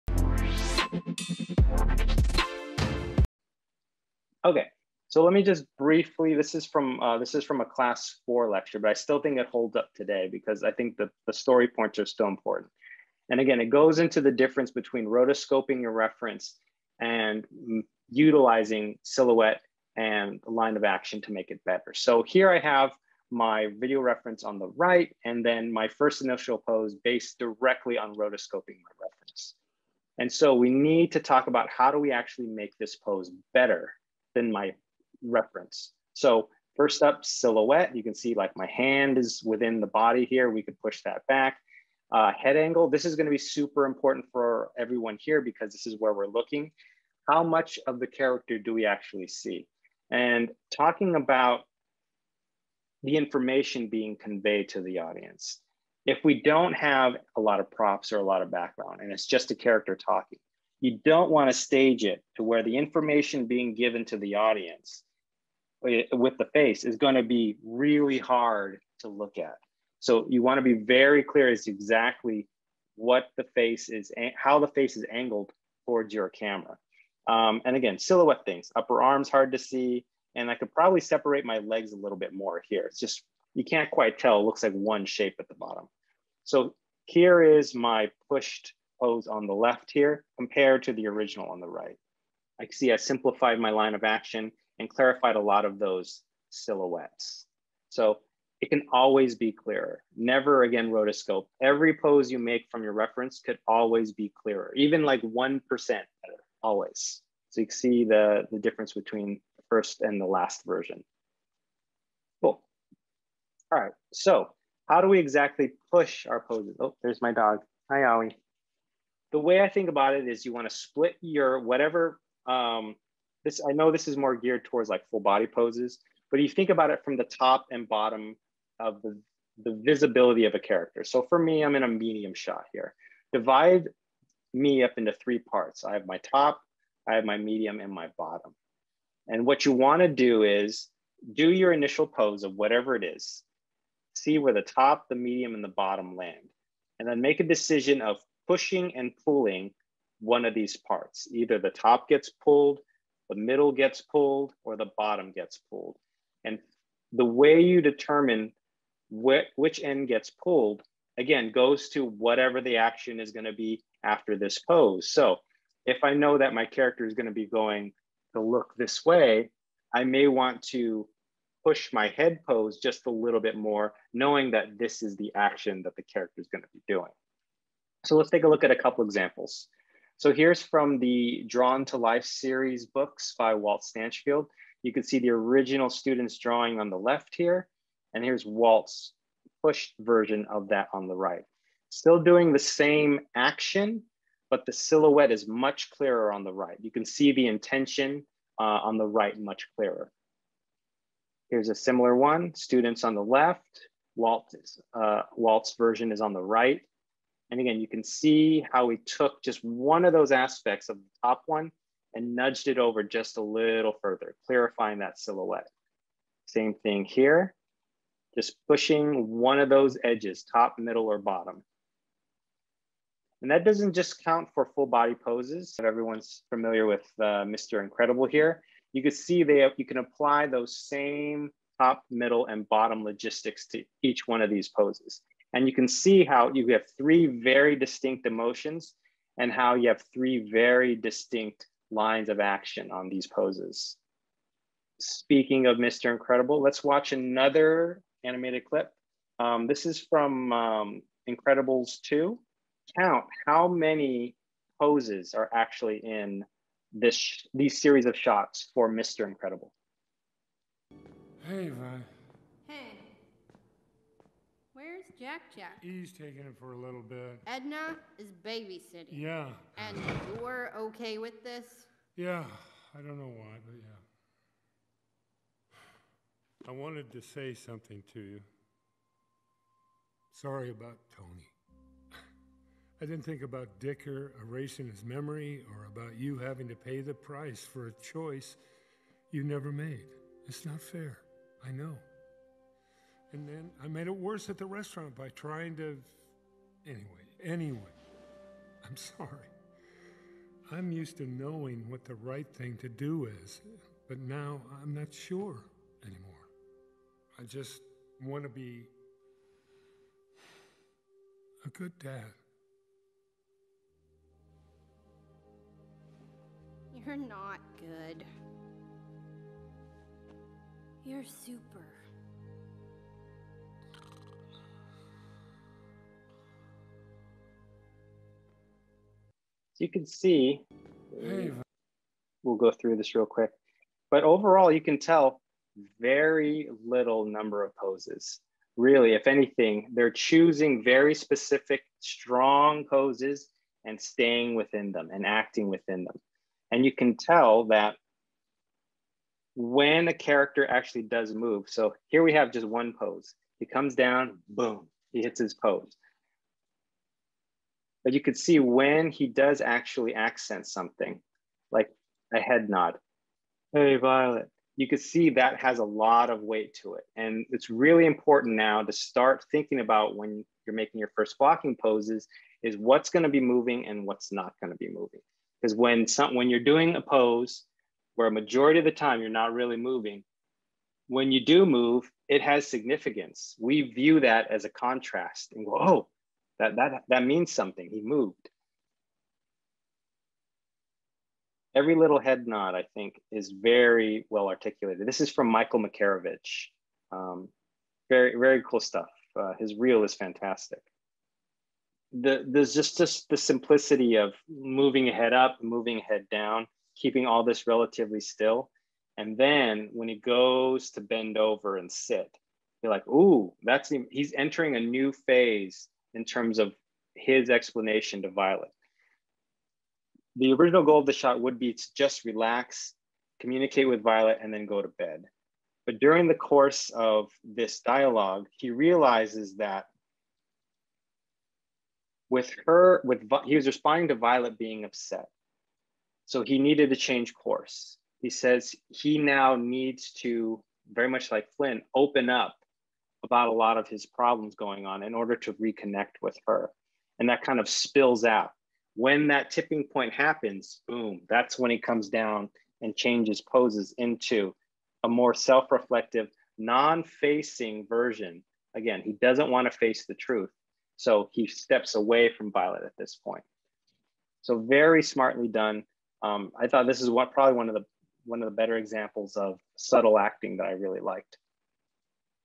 How to Push Your Animation Poses
AnimSchool Tips: How to Push An Animation Pose

In this lesson, AnimSchool instructor Garrett Shikuma demonstrates how to move away from rotoscoping your video reference to instead pushing your character’s silhouette and line of action to be better than the reference. Though Garrett says you can always push your poses, he emphasizes the importance of remembering to make intentional acting decisions while posing your character.

Join other 3D artists and animators in our online accredited courses. (ACCSC)
Reserve your spot in AnimSchool's next 11-week term at animschool.edu⁠